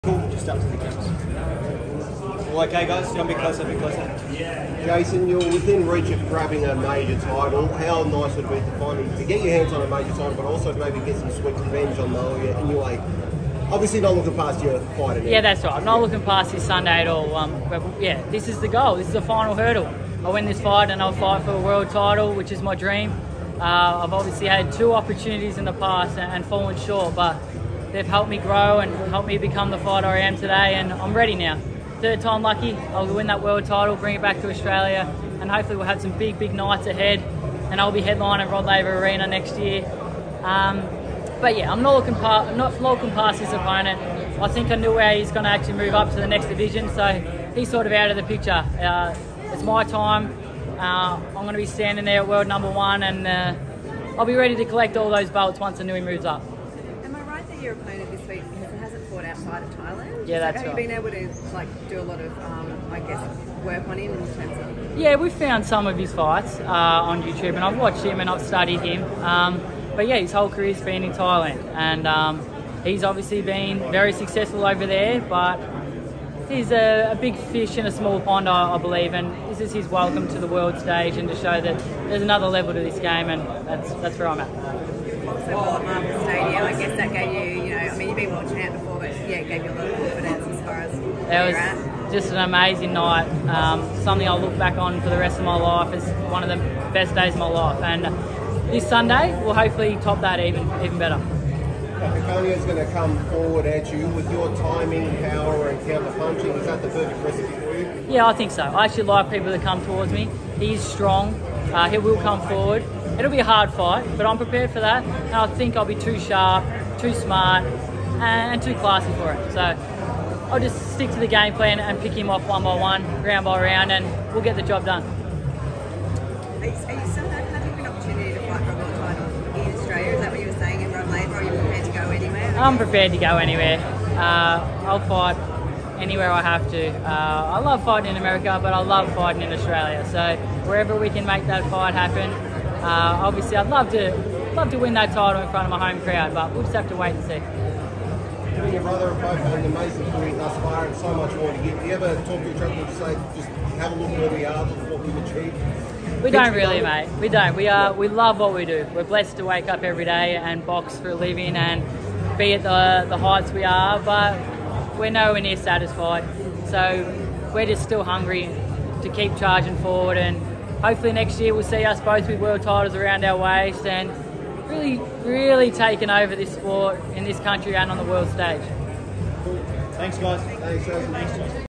‘I’LL BE READY TO COLLECT THOSE BELTS’ | JASON MOLONEY EYEING WORLD TITLE SHOT!
Jason Moloney speaks to the media ahead of his fight against Nawaphon Kaikanha on the undercard of Devin Haney vs George Kambosos Jr.